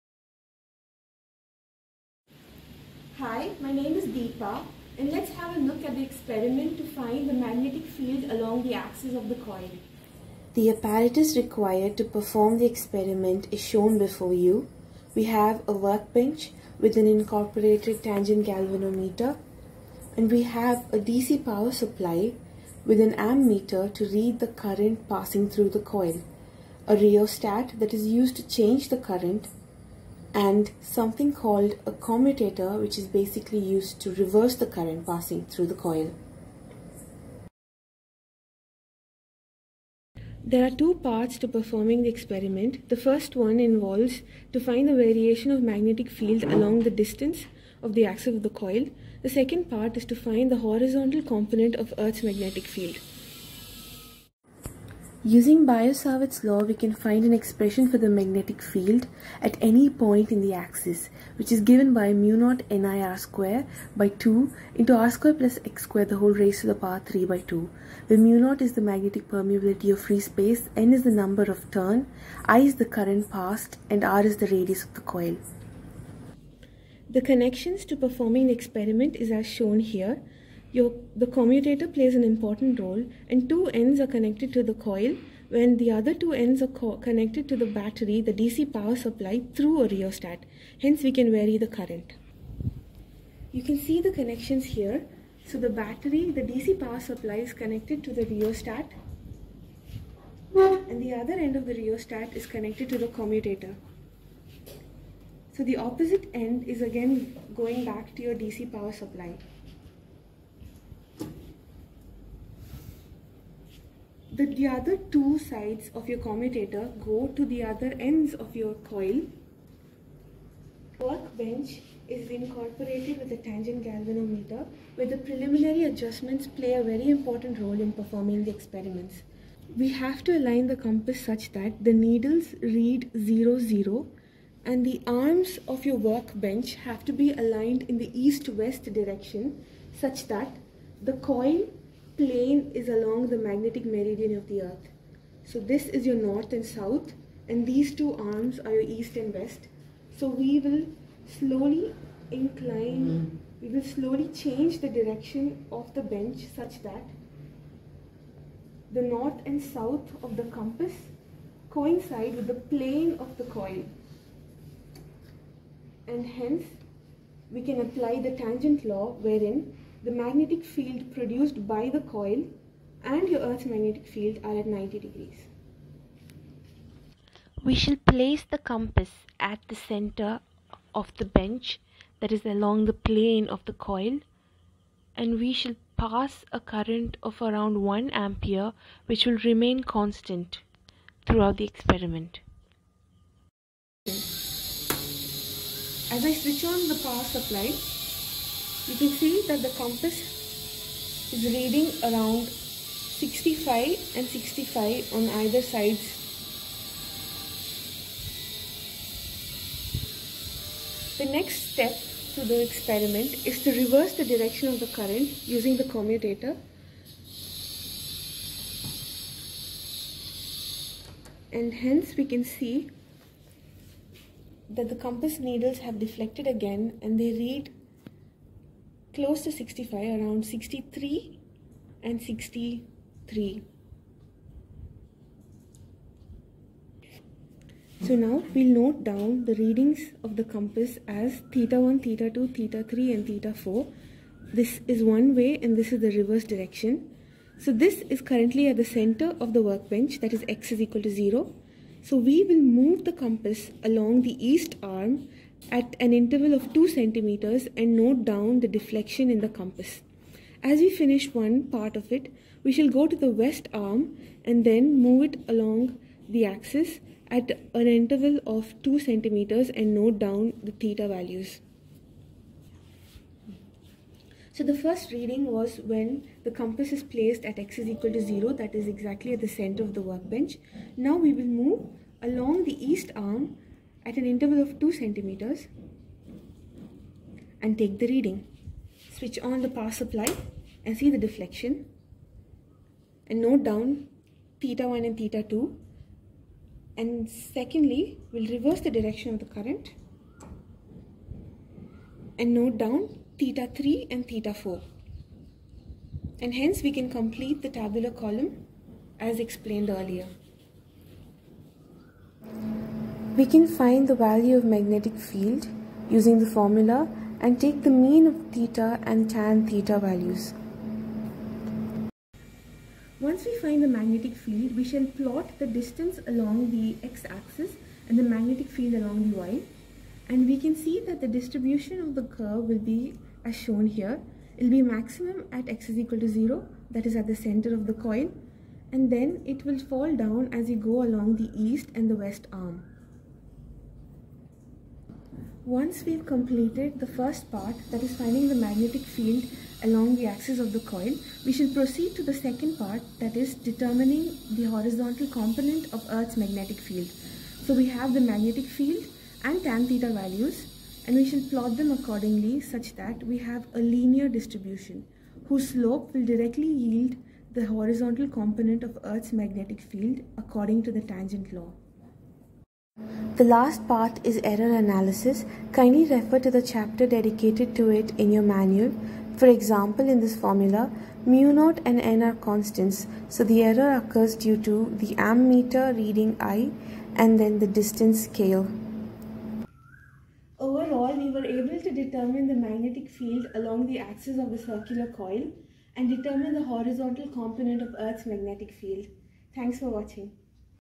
PH1030 - Magnetic Field
This is the video which goes with the experiment "Magnetic field" in the PH1030 lab course.